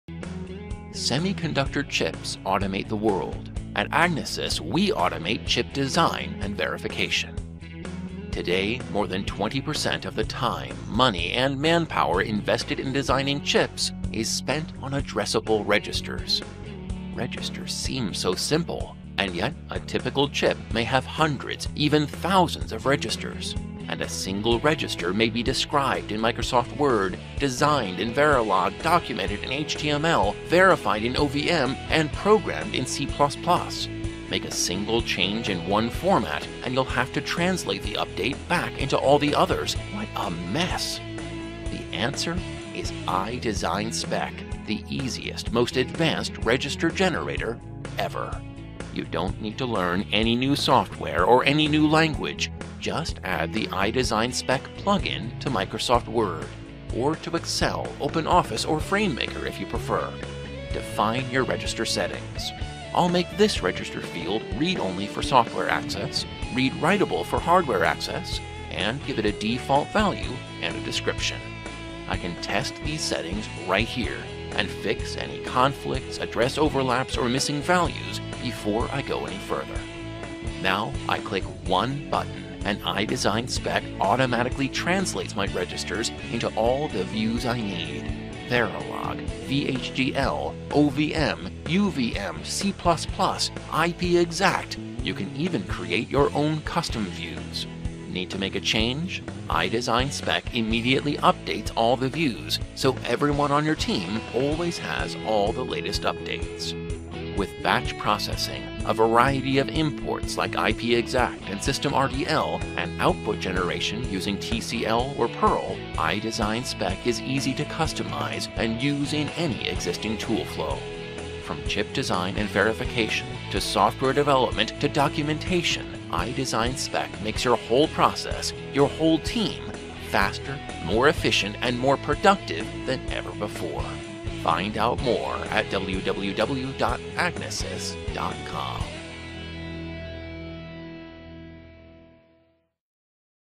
IDesignSpec : Register Generator
IDesignSpec is a Register Generator tool that enables you to quickly generate high quality code right from your document editor.
Outputs can be : Synthesisable RTL (Verilog, VHDL), verification code (UVM, OVM, VMM, eRM), IP-XACT, SystemRDL, etc.
It can be installed in Word, Excel, OpenOffice or FrameMaker.
It can import IP-XACT, SystemRDL and it is available as a batch program on Windows and Linux OS.